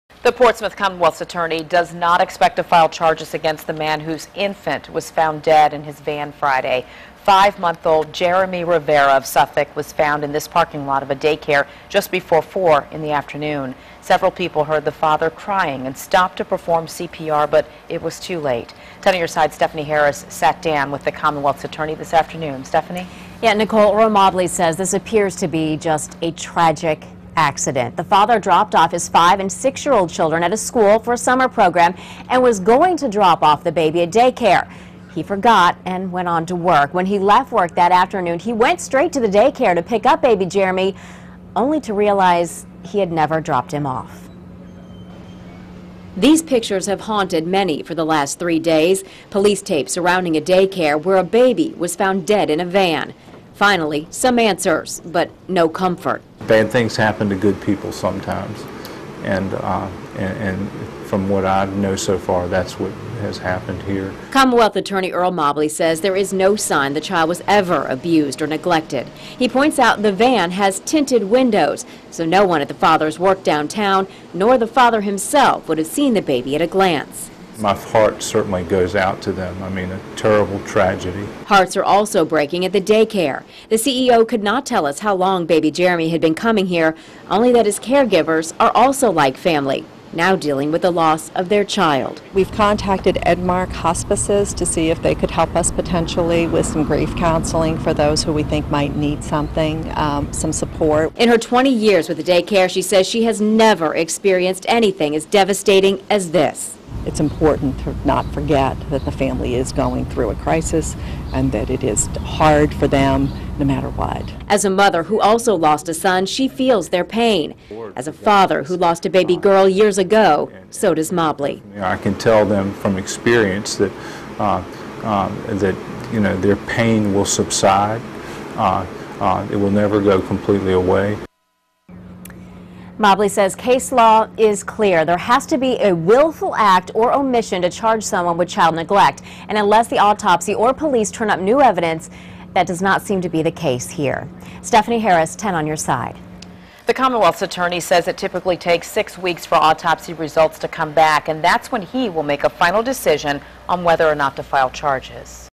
5-month-old infant found dead in father's car
5-month-old infant found dead in father's car